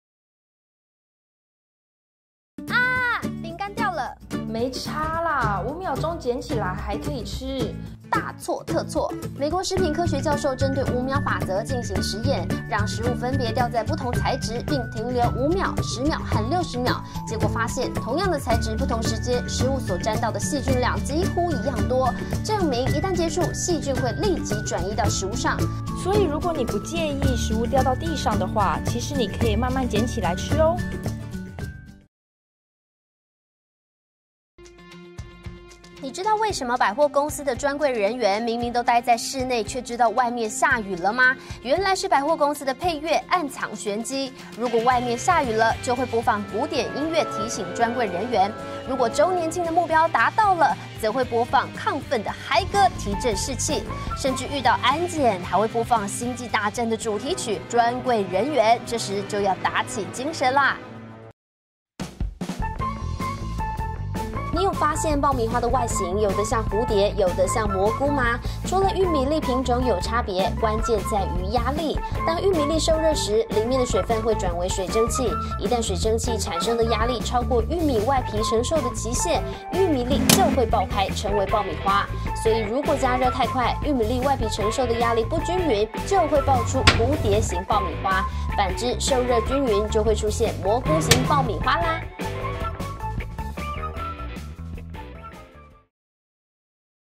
【冷知識】實驗告訴你，食物到底掉到地上多久撿起來還能吃？
食物掉到地上快速撿起來可以吃嗎？
為什麼專櫃人員知道外面下雨了？
爆米花怎麼爆出蝴蝶型和磨菇型的呢？